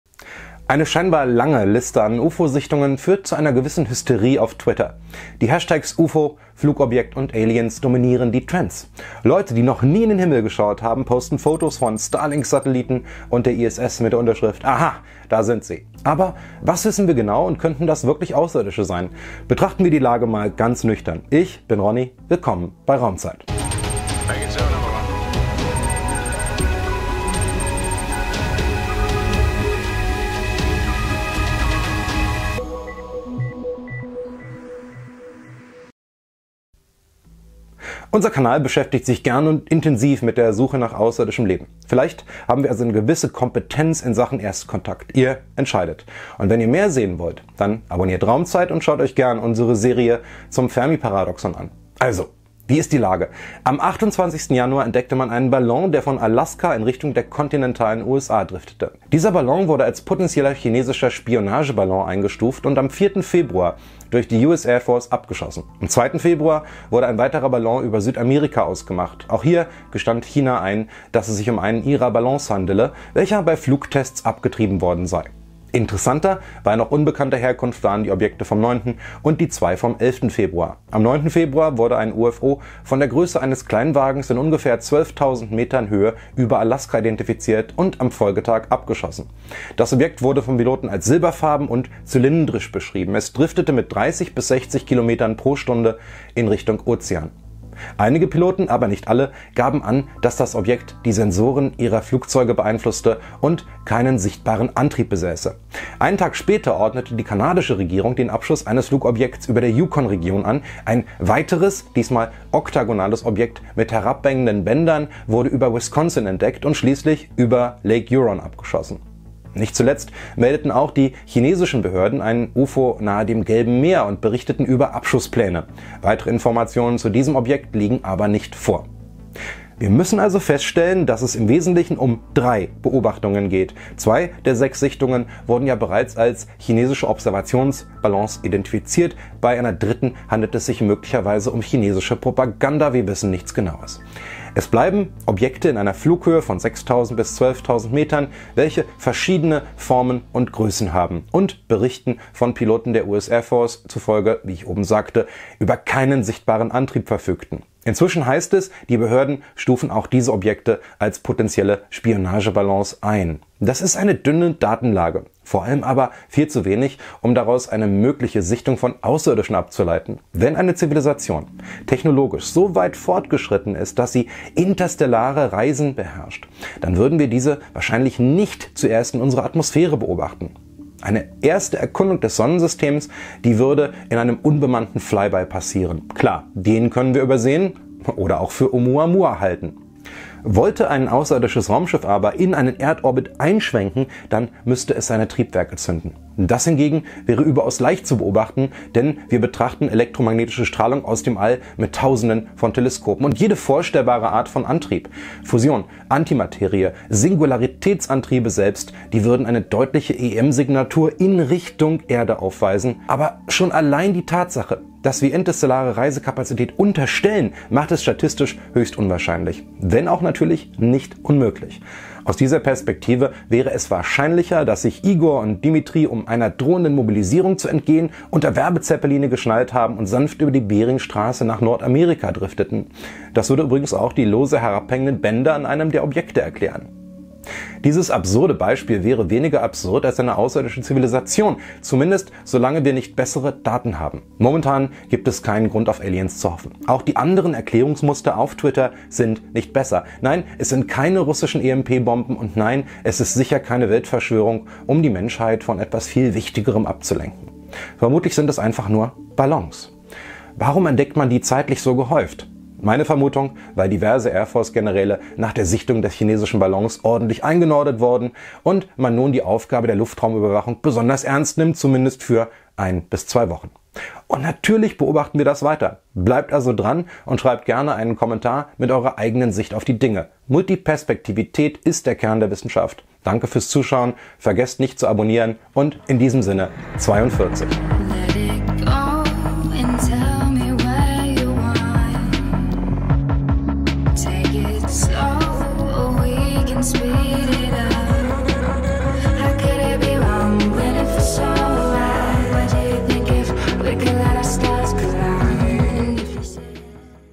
UFO Sichtungen überall. Was verbirgt sich dahinter? Raumzeit (2023)
Immer neue Sichtungen von nicht identifizierten Flugobjekten haben einen gewissen UFO-Hype in den sozialen Medien ausgelöst. Was wissen wir eigentlich genau – und können das Aliens sein?

Staffel 6 | Episode 04

Direkt zu eurem Inhalt:

00:00 Intro
00:42 Hauptteil

Raumzeit bietet euch wöchentlich spannende Einblicke in das Universum, die Physik und in die Zukunft der Menschheit. Gelegentlich diskutieren wir auch gesellschaftliche Themen.  🌎